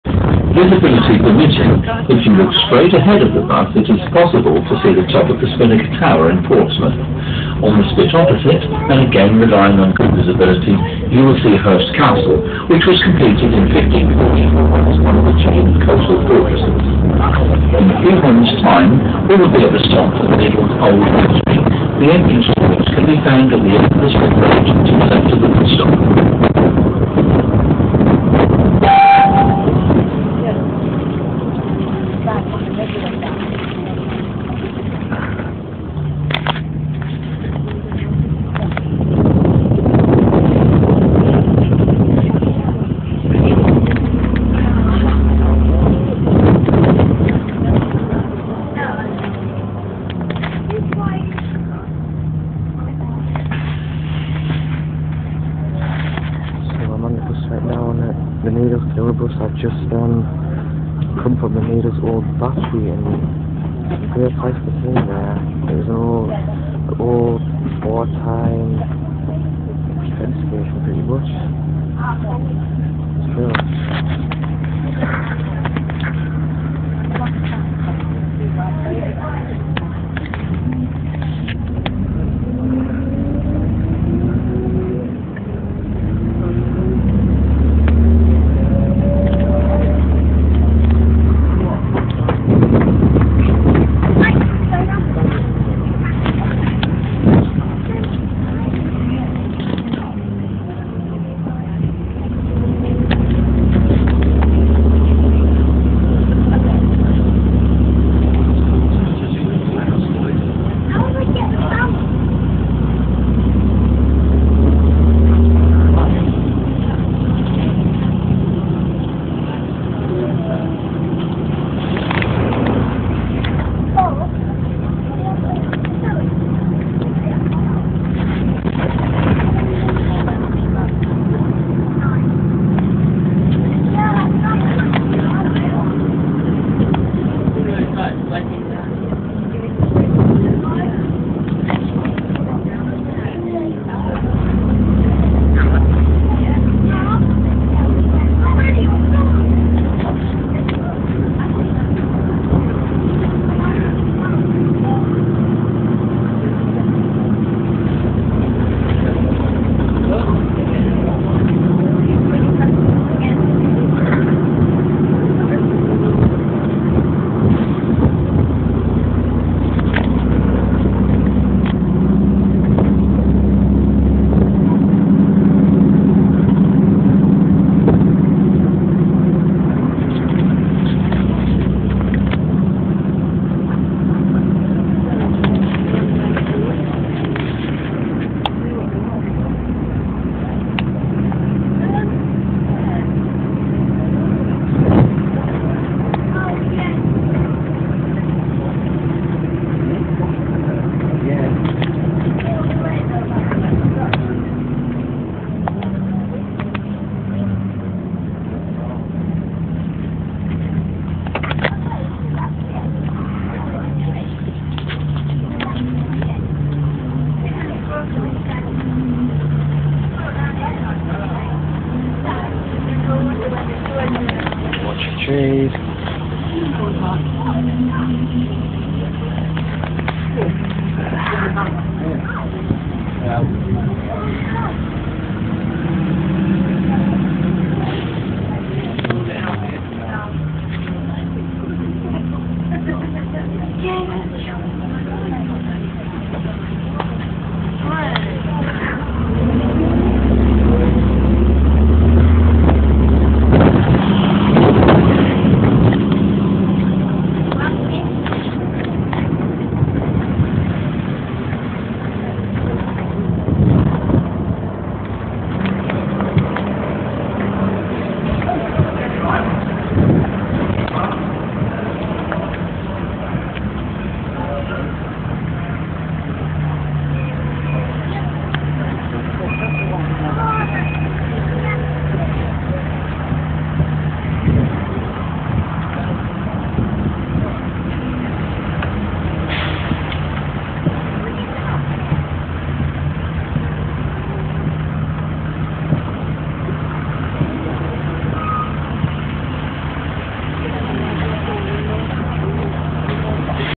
The Needles Tour 2
Sights and sounds from the Needles open top bus tour.